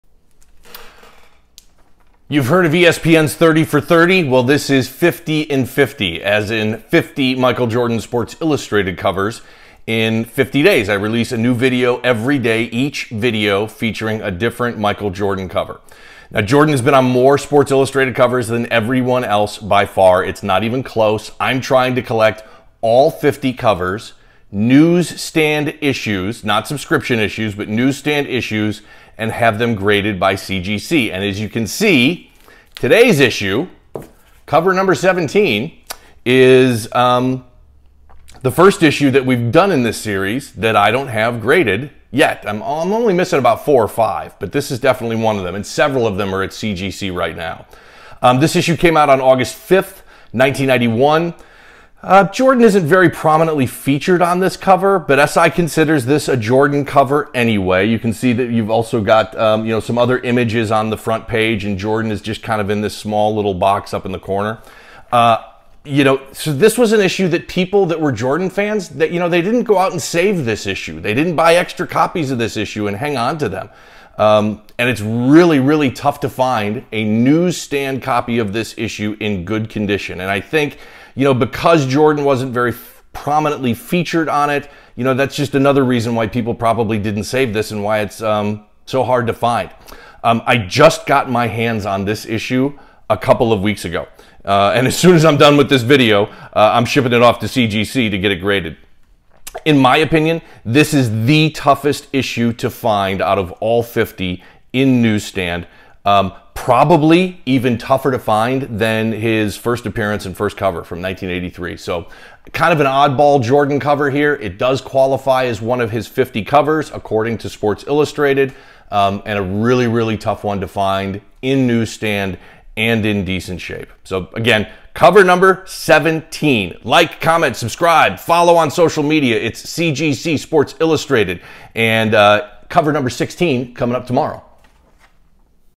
Jordan 50 in 50 - Cover #17
I'm featuring all 50 Michael Jordan Sports Illustrated covers.  This episode is cover #17

The who, what, when where and why of collecting CGC Sports Illustrated issues.